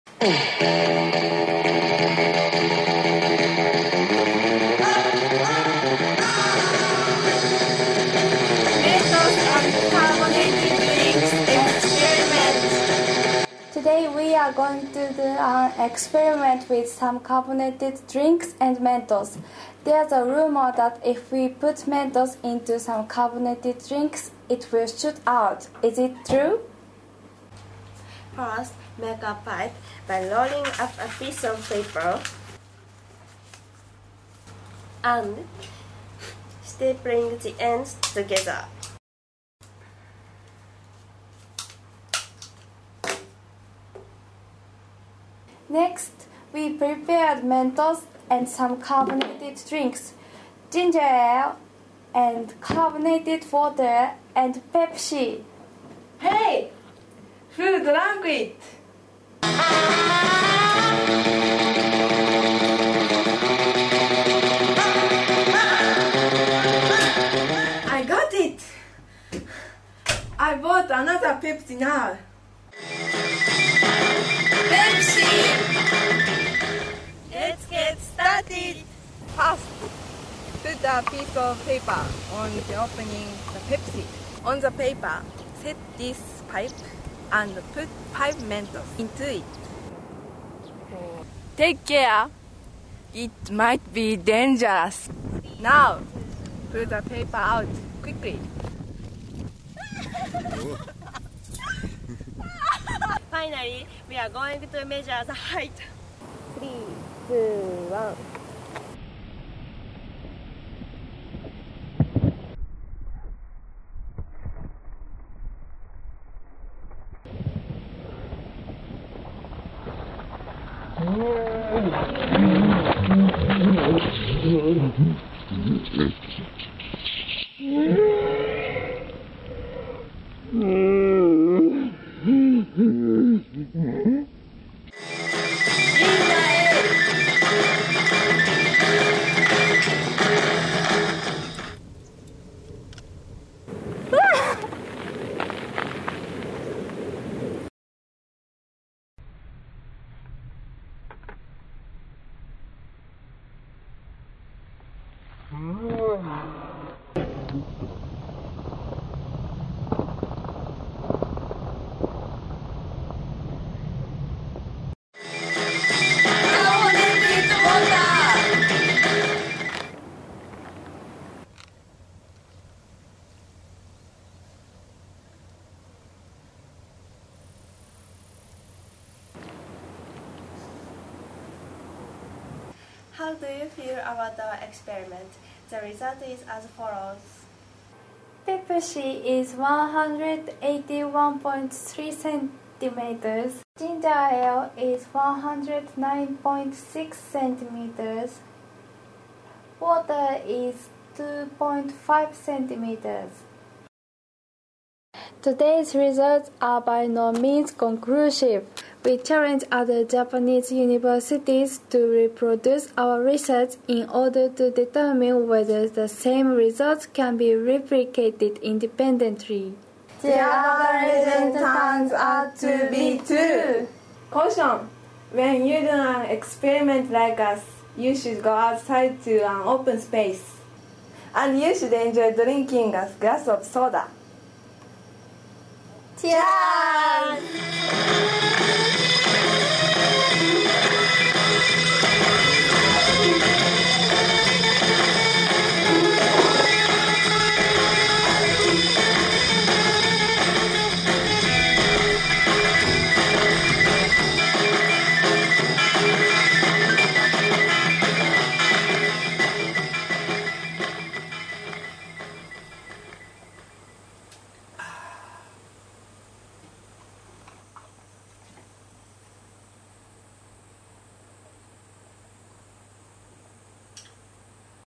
Mentos and Carbonated Drinks Experiment
An urban legend is verified (Part 1). What if we put Mentos into some carbonated drinks? 都市伝説検証パート１：炭酸飲料にメントスをいれると爆発するのか？